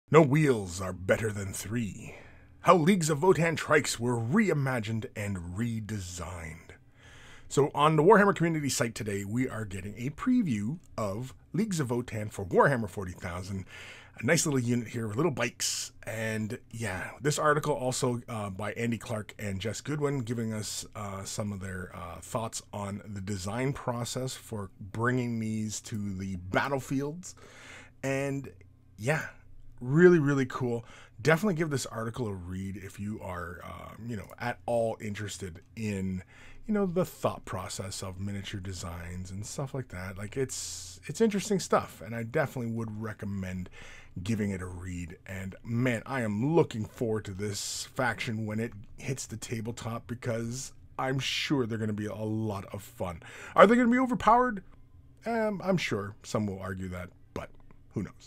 No Wheels Are Better Than Three How Leagues of Votann Trikes Were Reimagined and Redesigned
In this Warhammer Community article we get some insight into the design process and thoughts from the design team at Games Workshop. The new Hernkyn Pioneers trike bikes are previewed in this Warhammer 40K table top game reveal.